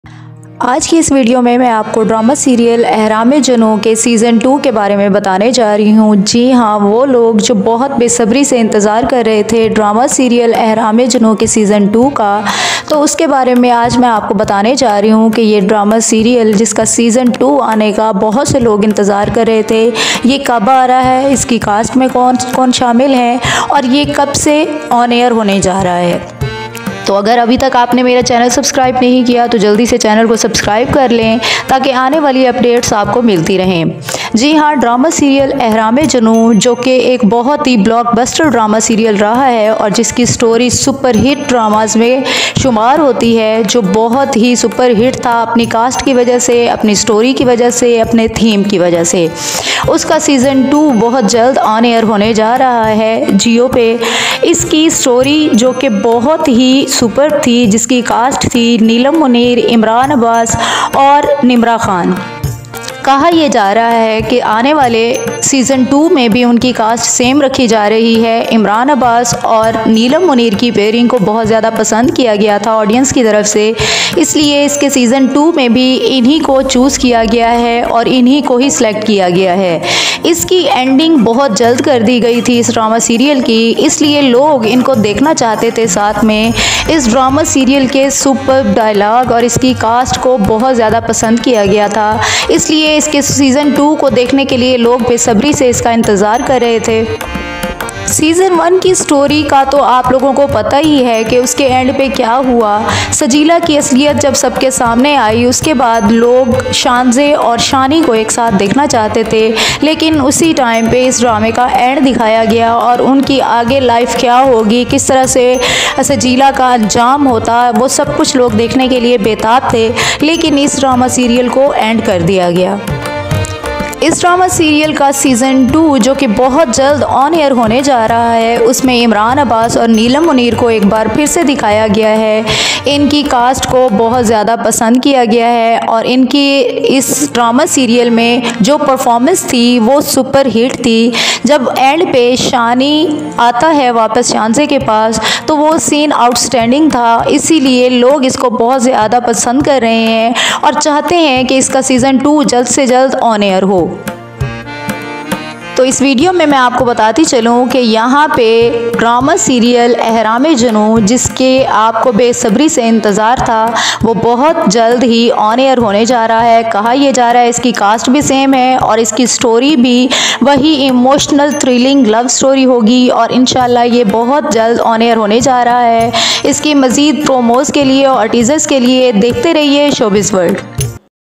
Ehraam-e-Junoon Season 2 Promo | Promo Review | Har Pal Geo Drama Ehraam-e-Junoon
In this video, we dive into the exciting promo review of Ehraam-e-Junoon Season 2, the latest hit drama from Har Pal Geo! Join us as we explore the captivating storyline, character developments, and the stunning visuals that have fans buzzing with anticipation. Discover what makes this season a must-watch and why it continues to dominate the ratings. From romantic twists to dramatic turns, Ehraam-e-Junoon Season 2 promises to deliver an unforgettable viewing experience. Don't miss out on our detailed analysis and insights! Tune in now for all the latest updates and join the conversation in the comments below!